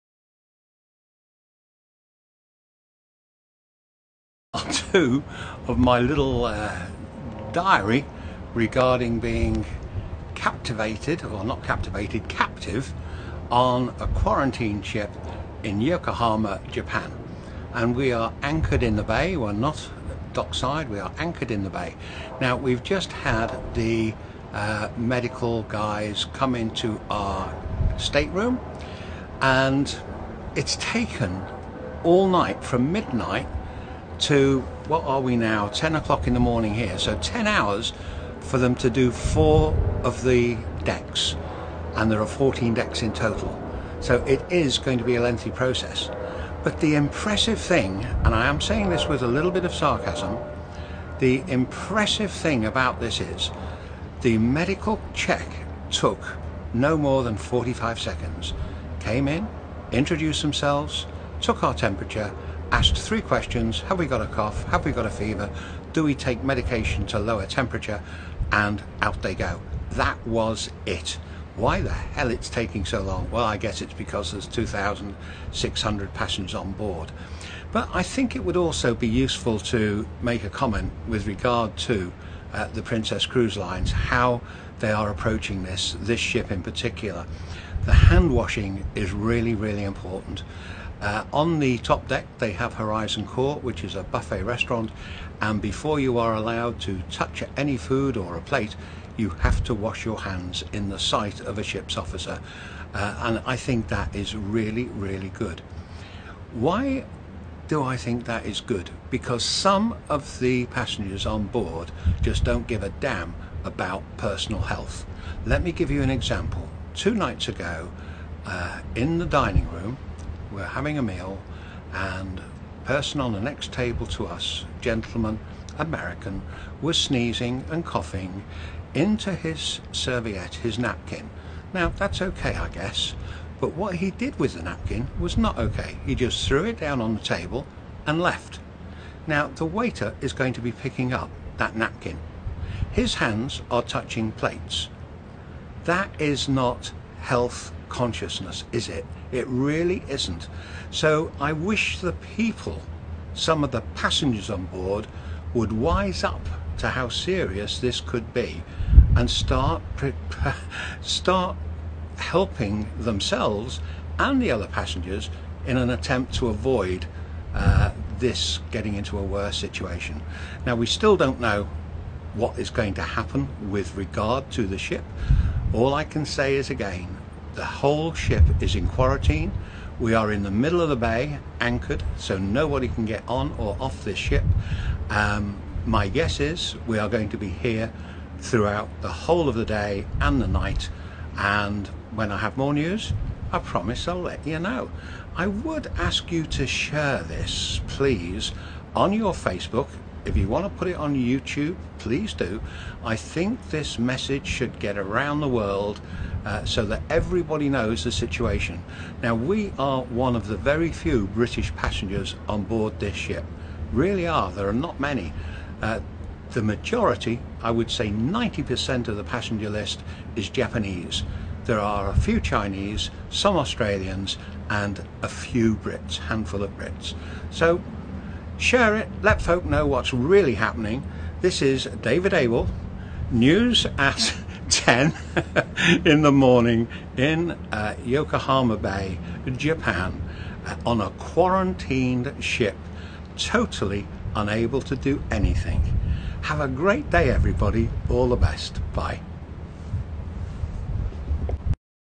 Diamond Dave The REAL David Abel in Quarantine on Diamond Princess Video 1.
DiamondDave TheREAL_DavidAbel in Quarantine on Diamond Princess Video 1.
First testing of passengers health. Hygiene of paramount importance.
Diamond Dave rants over the way one passenger disregards the health of passengers around him.
Anchored in Yokohama Bay.